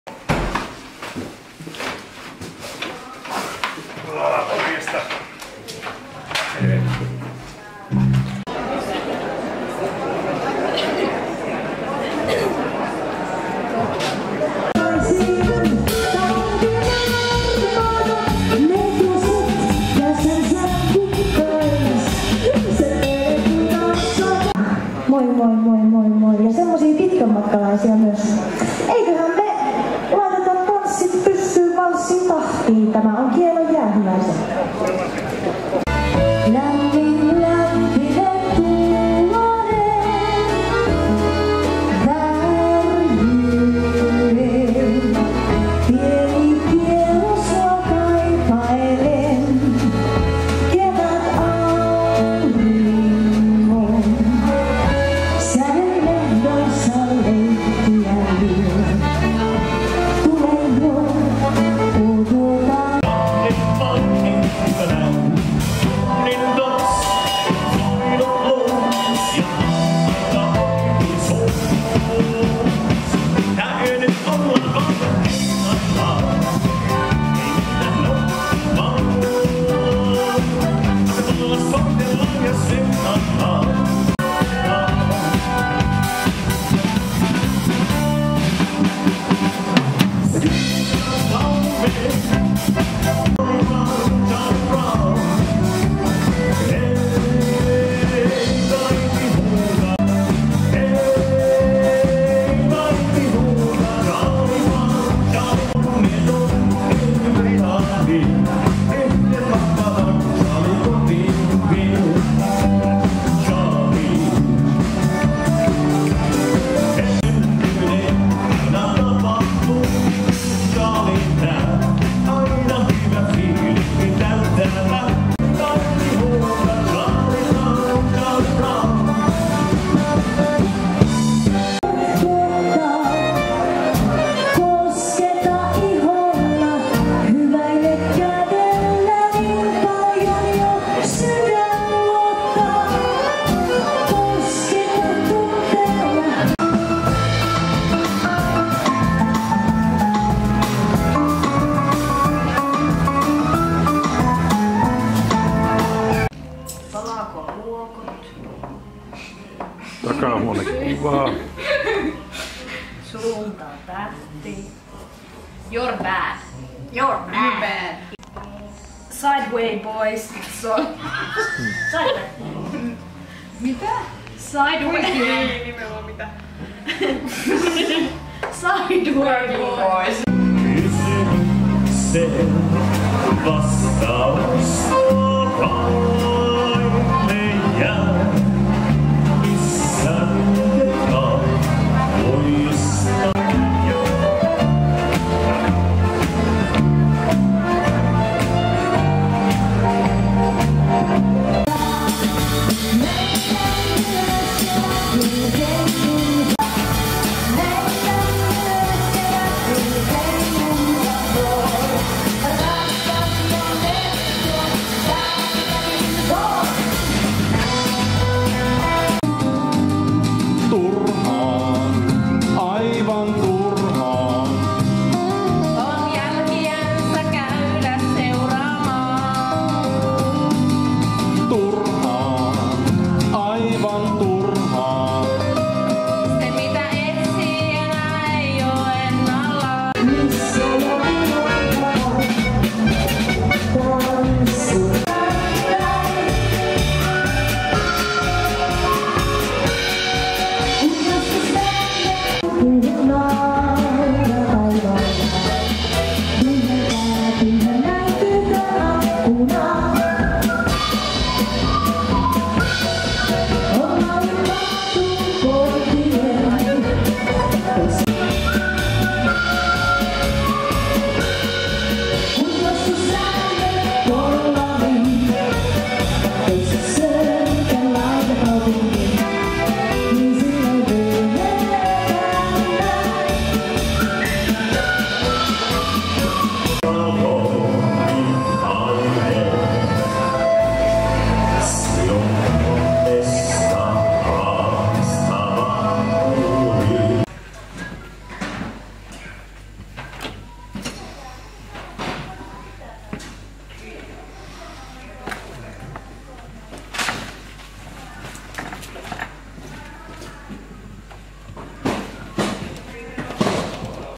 Syksyn Suurtansseista
Syyskuun Suurtanssit Naapurinvaarallla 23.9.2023. Esiintymässä Tanssiorkesteri Neitoset ja Jyrki Nurminen&Sävel